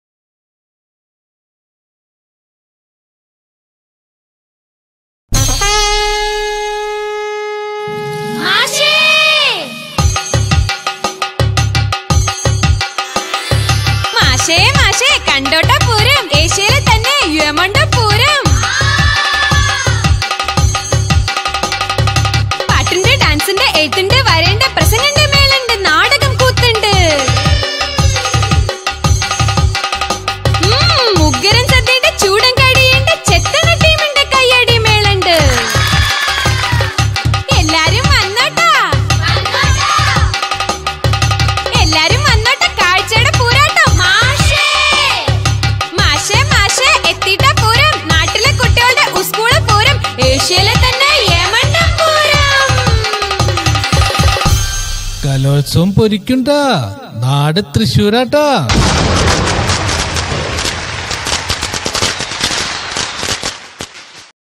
കേരള സ്കൂൾ കലോത്സവം തൃശൂരില്‍ ജനുവരി 6 മുതല്‍ 10 വരെ
കലോത്സവത്തെ വരവേല്‍ക്കാന്‍ തൃശൂരുകാരുടെ പാട്ട്...
പാട്ട് പൊരിച്ചൂട്ടാ ട്ടാ .ഉത്സവ കാലം വീണ്ടും .കേരള സ്കൂൾ കലോത്സവം തൃശൂരില്‍ ജനുവരി 6 മുതല്‍ 10 വരെ..